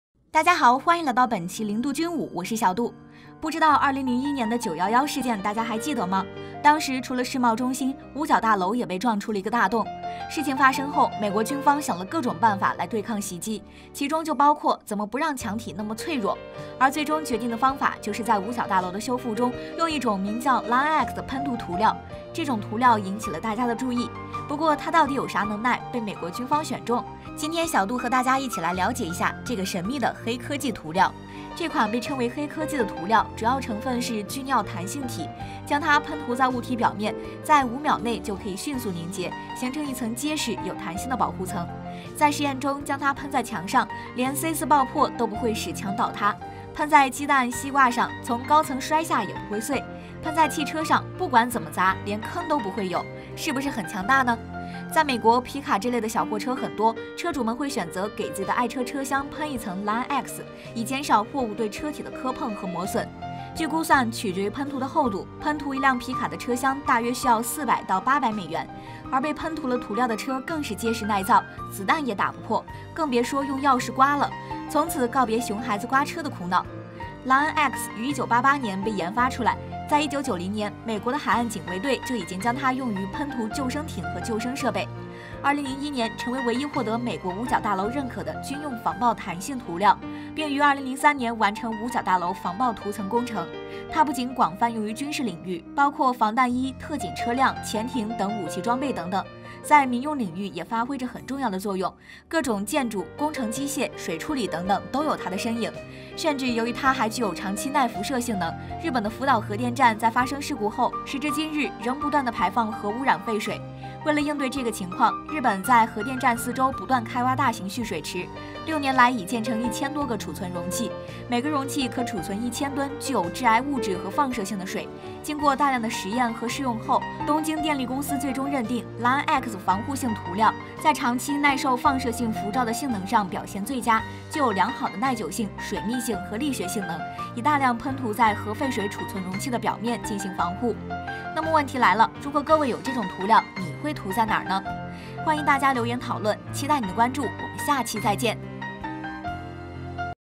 五角大楼换上黑科技涂层，喷层涂料就防弹防爆防辐射，你家车也行
911之后，五角大楼防爆层换上一层黑科技涂层，喷层涂料就能做到防弹防爆防核辐射。汽车、小船、防弹衣也可以通过喷这种涂料达到同样的效果。